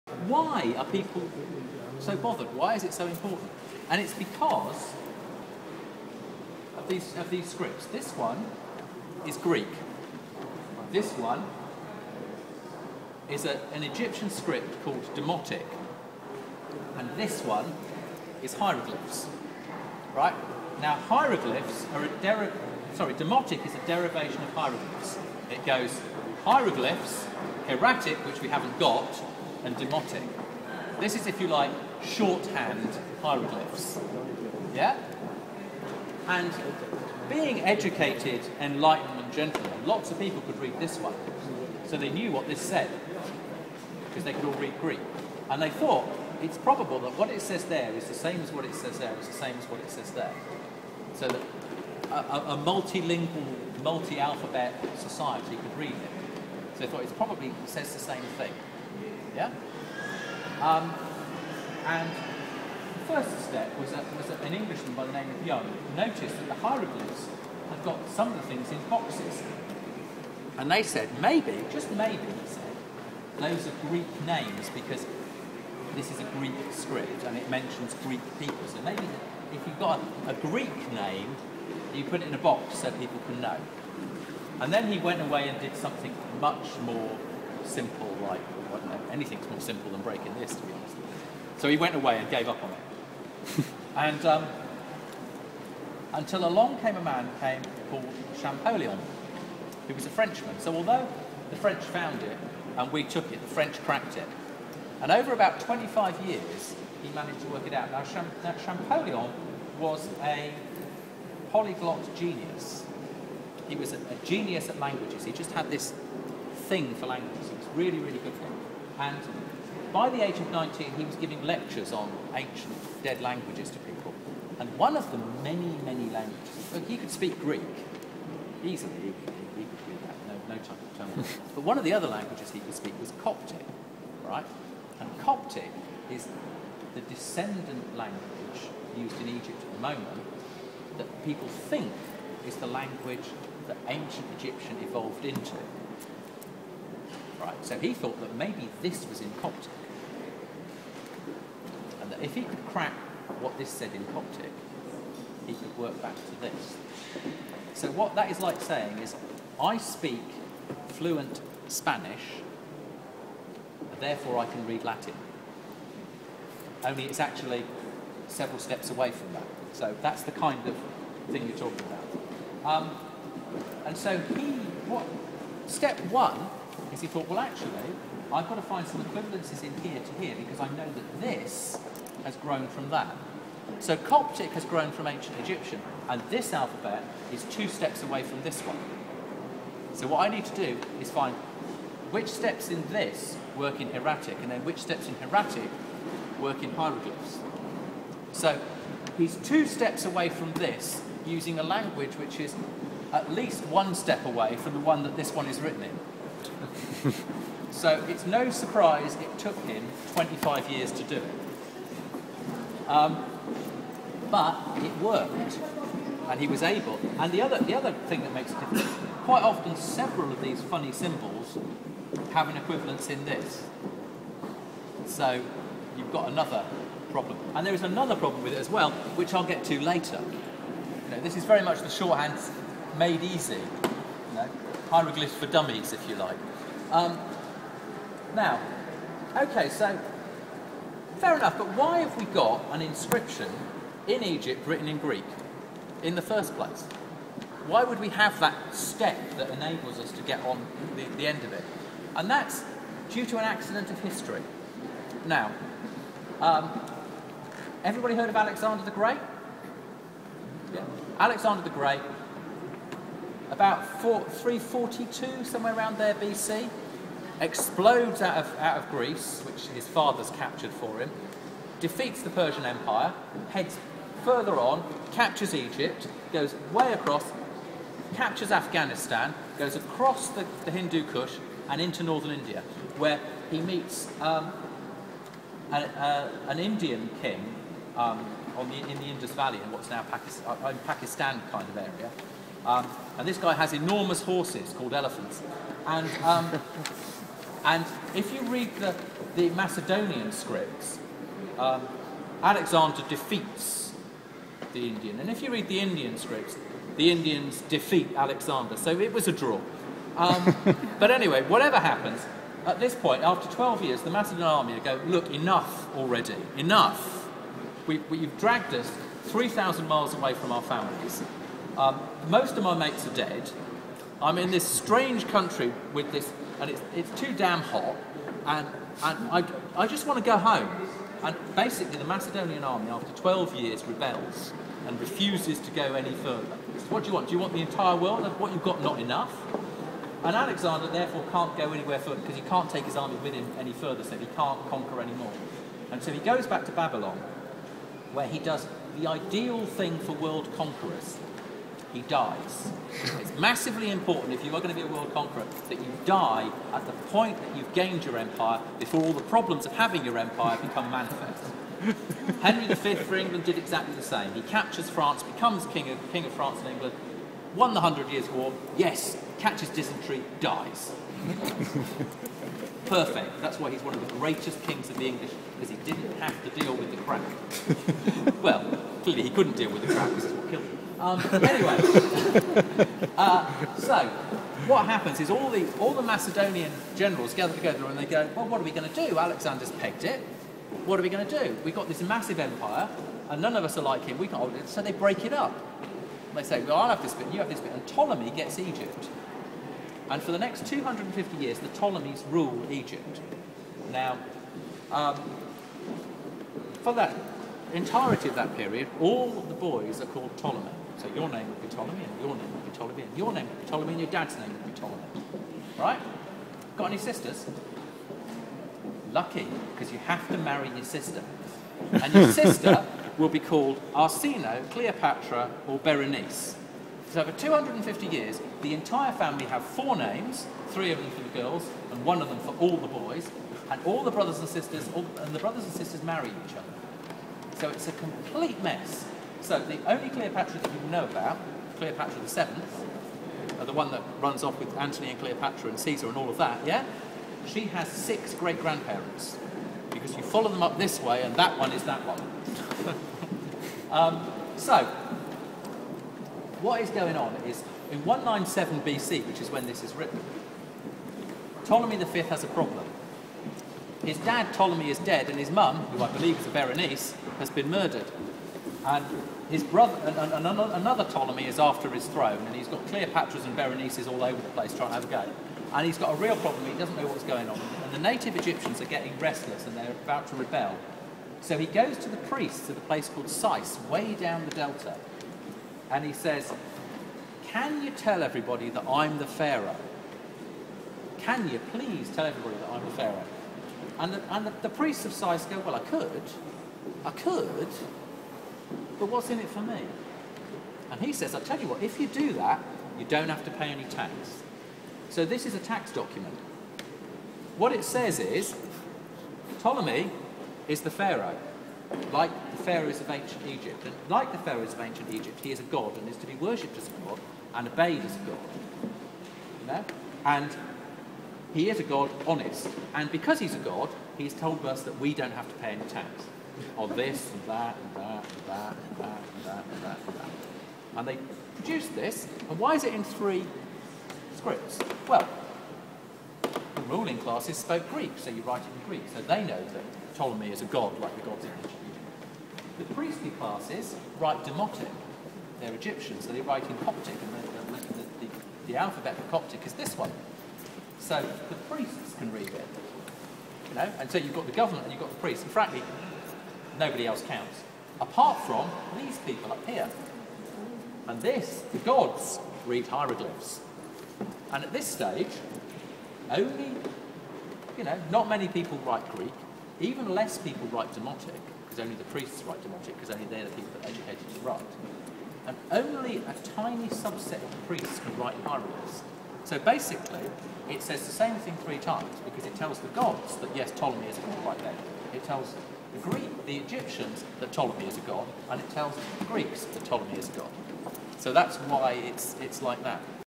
Rosetta Stone Explanation at the British Museum
An explanation by a great tour guide on the importance of the Rosetta Stone